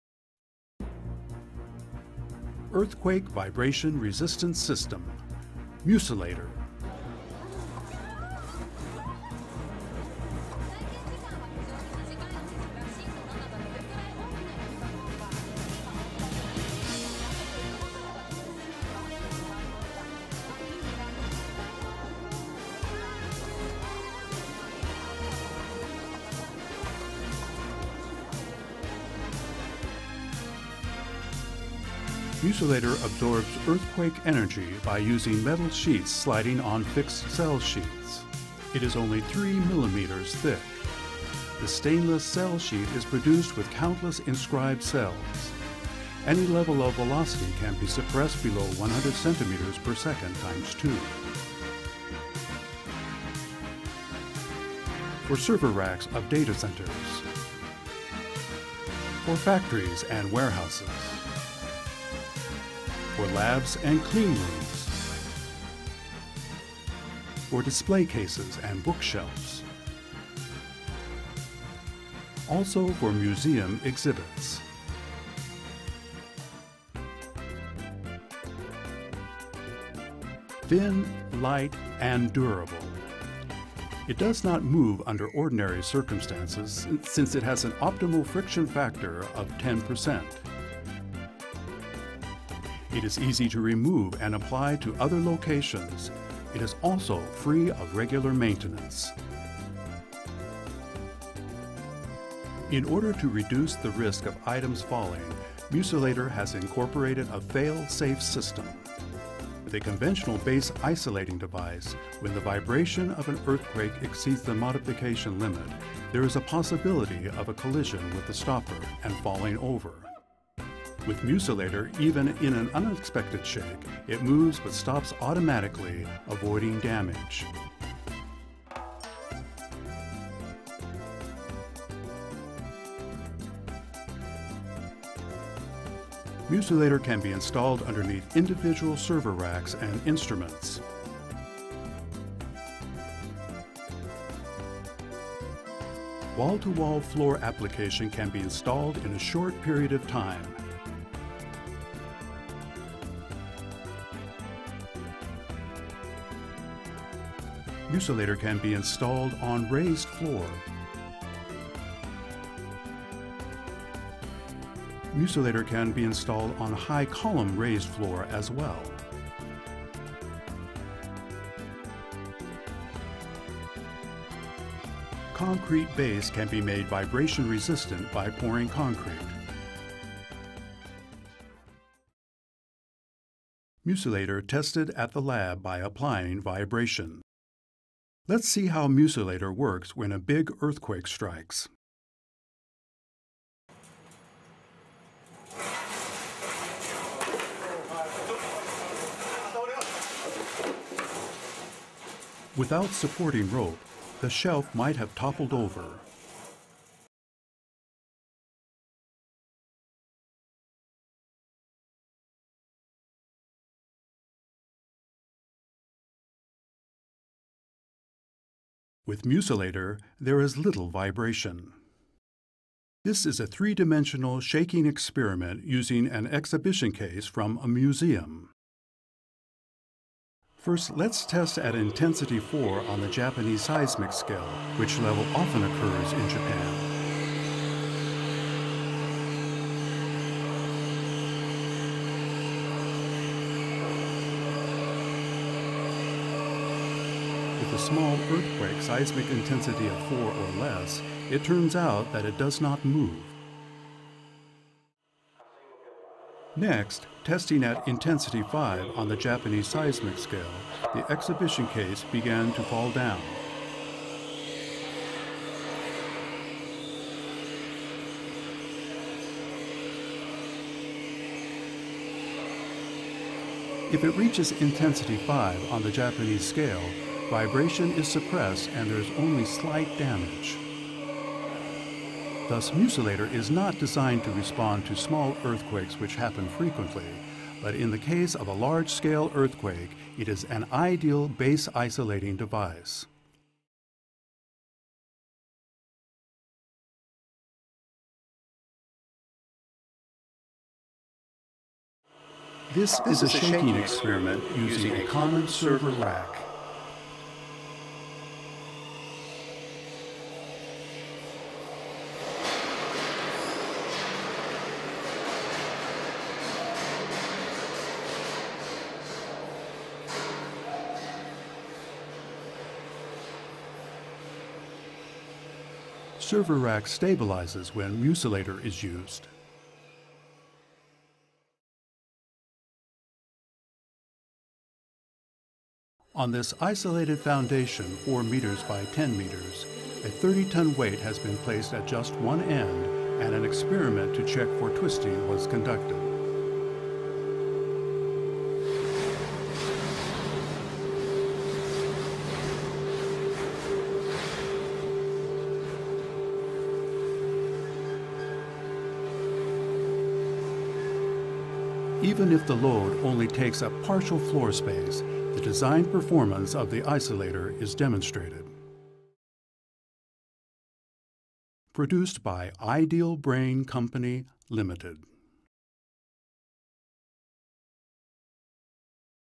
This movie trailer shows μ-Solator:Product information
Product information
and vibration experiments

Product introduction of earthquake vibration resistant system μ-Solator.
The movie trailer will show vibration experiments and various application cases.